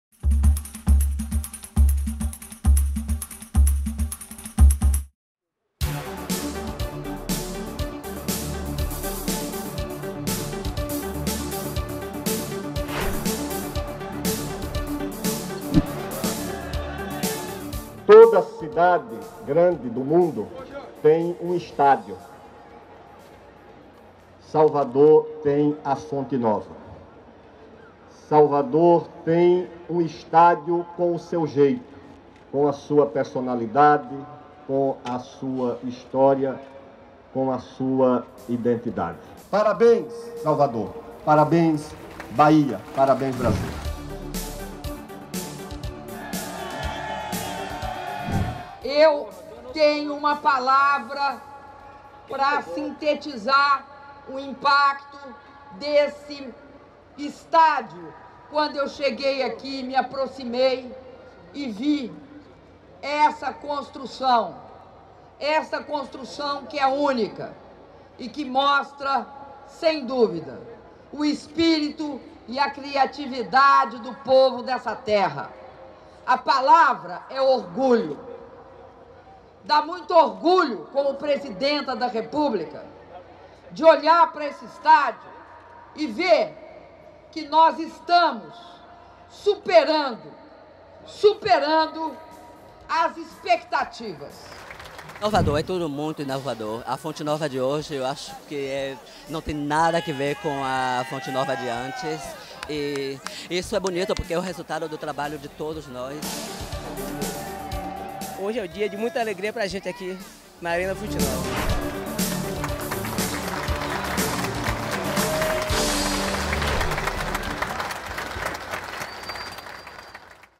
Entrega oficial da Arena Fonte Nova
Vídeo sintetiza a cerimônia de entrega da Arena Fonte Nova, em Salvador, com a presença da presidenta da República, Dilma Rousseff, do ministro do Esporte, Aldo Rebelo, além de autoridades locais e operários que fizeram parte da obra. O estádio baiano receberá três jogos da Copa das Confederações de 2013 e seis partidas do Mundial de 2014. Saiba mais sobre a preparação brasileira para 2014 no Portal da Copa